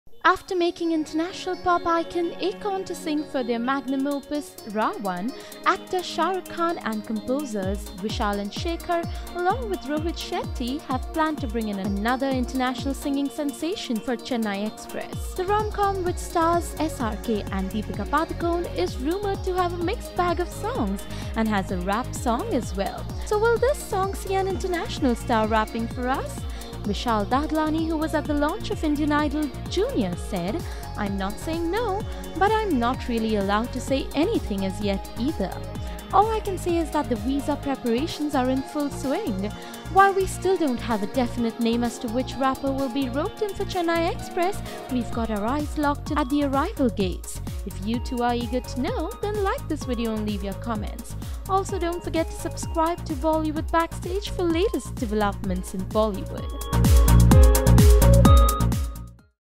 Chennai Express to feature international singer!
Chennai Express to feature international singer - After roping in international singer Akon for RA.One, Shahrukh Khan is soon going to feature another international sensation in his upcoming movie, Chennai Express. Find out who?